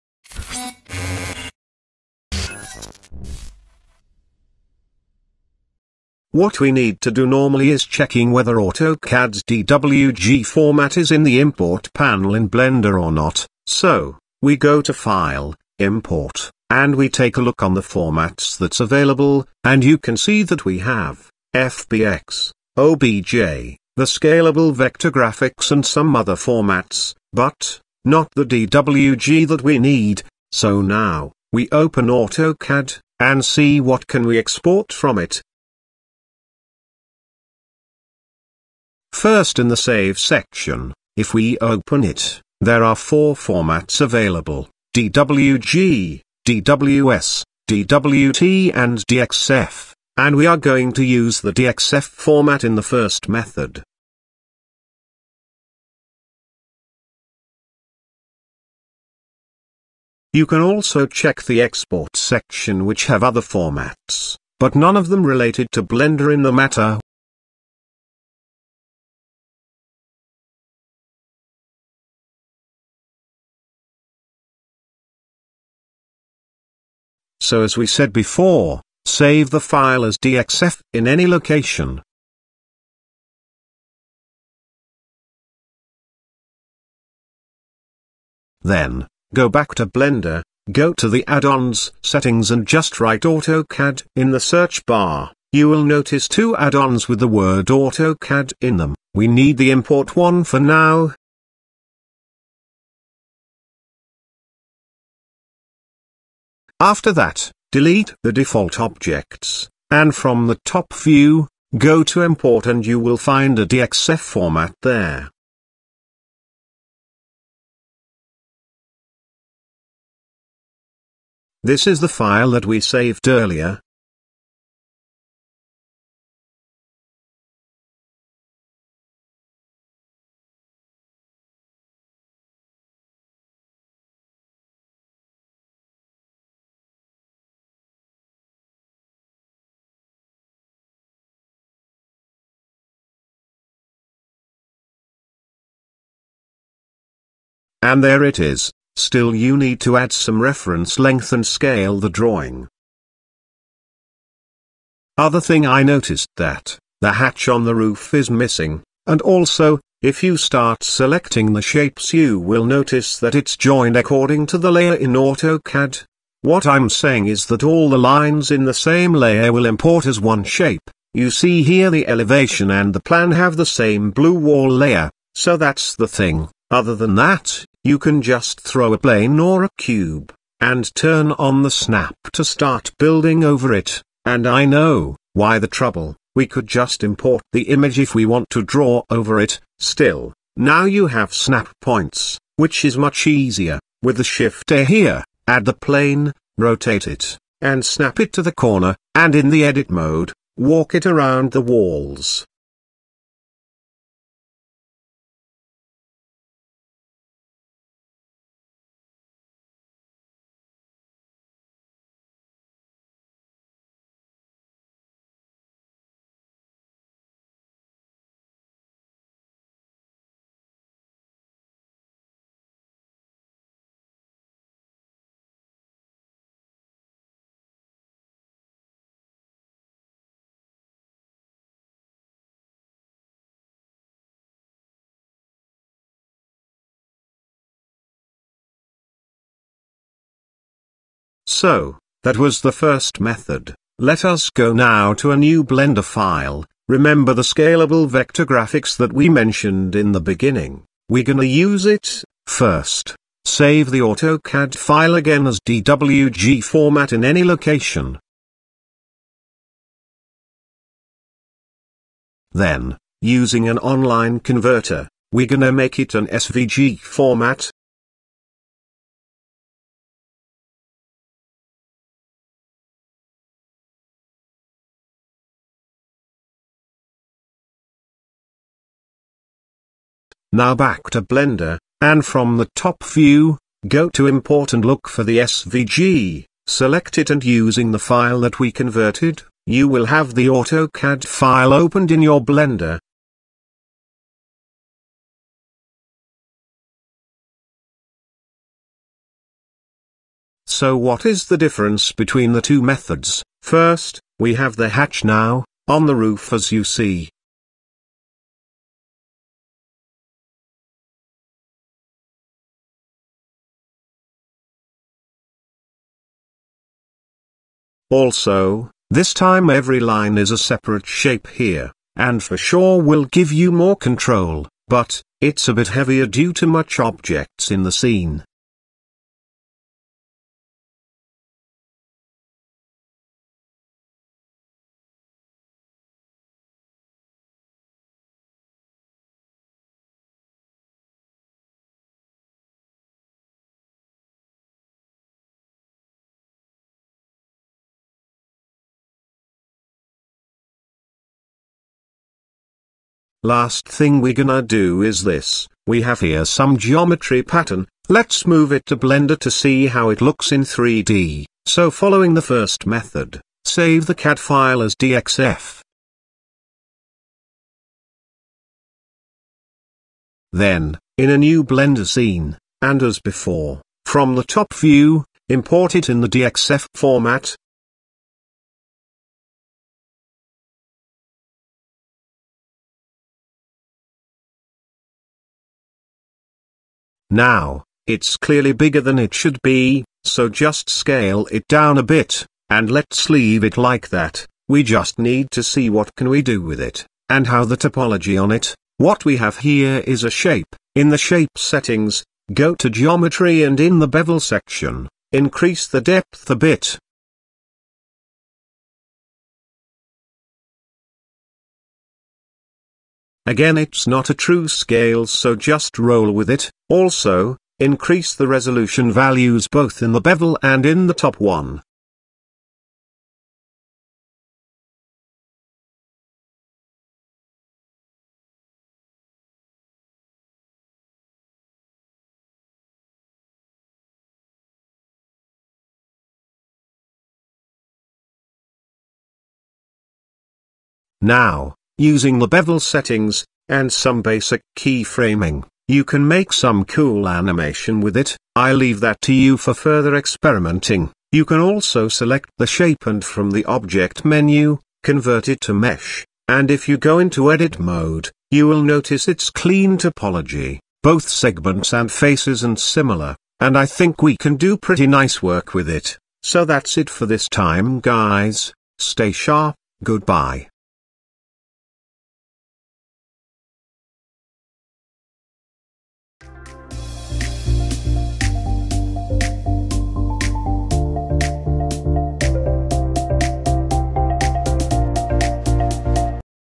How to import An AutoCAD Drawing to Blender | Blender Tutorial
In this Tutorial ,We will go through the methods of importing an full editable AutoCAD drawing into blender and the Difference between them.

introduction: (0:00)
Checking the formats available in both programs  : (0:06)
First method - Importing the file as DXF format : (01:17)
Second method - Importing the file as SVG format : (04:04)
Animations With Geometry Patterns : (05:53)
Outro: (07:56)